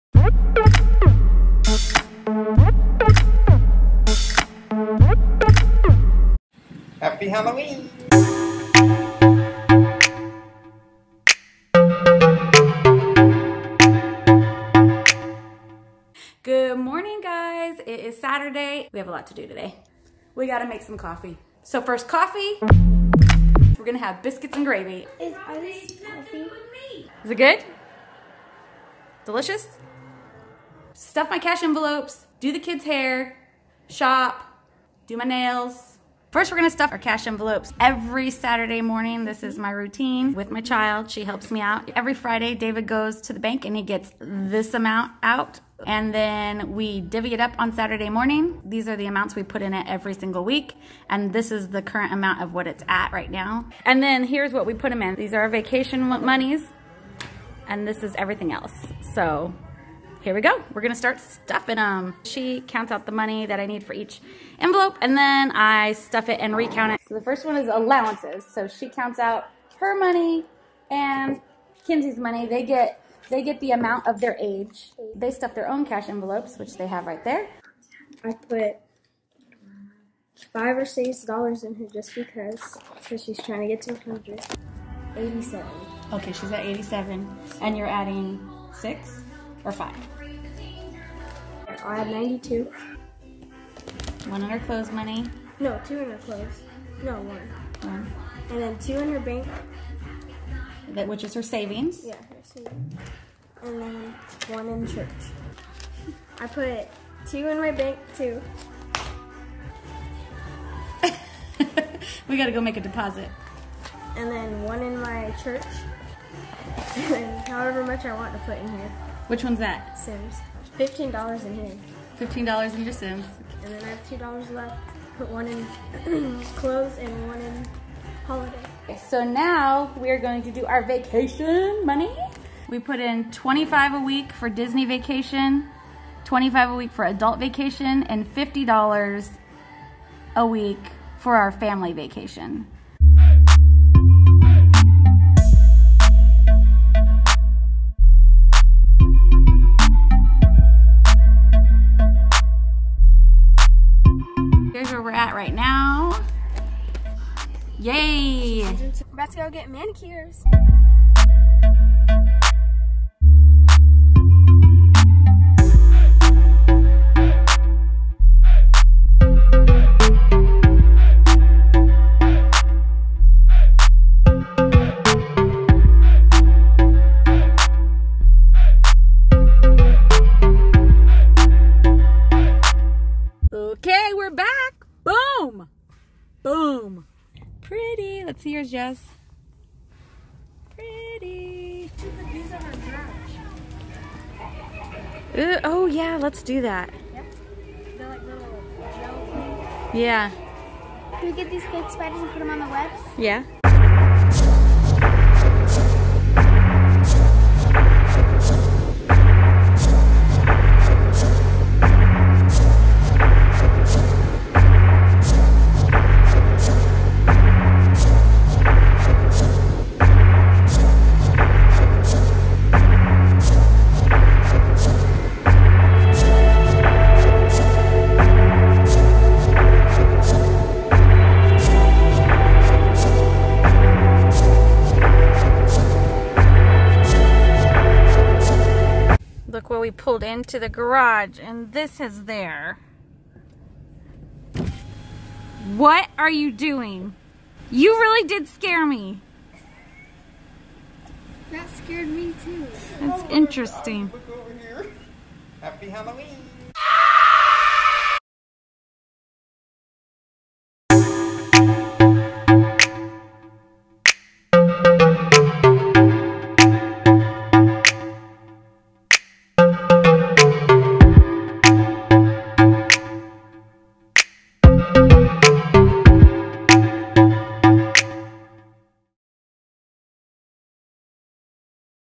Vlogging Everyday - Day 2 - DITL, Shop with Me, Budget with Me
Today we stuffed our cash envelopes, shopped for halloween decor, had a girls day at the nail salon, and did Kinzie's hair. It was a fun filled, but busy day! I hope you enjoy.